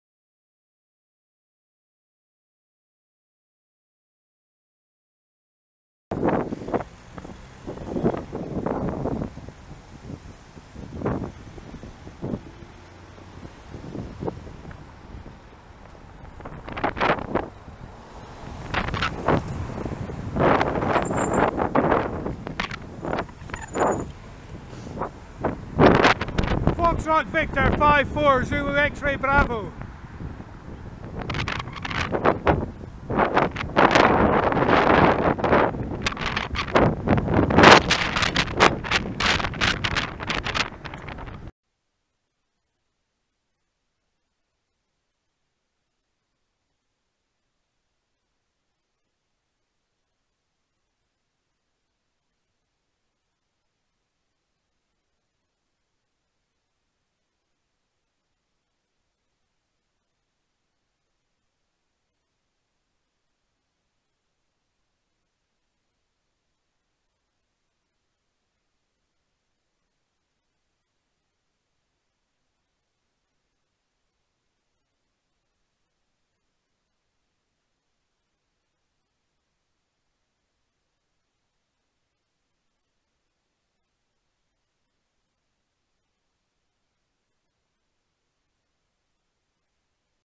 FV54ZXB close pass on zig zags
Reported to West Midlands Police via nextbase. I braked, so did they so I braked again.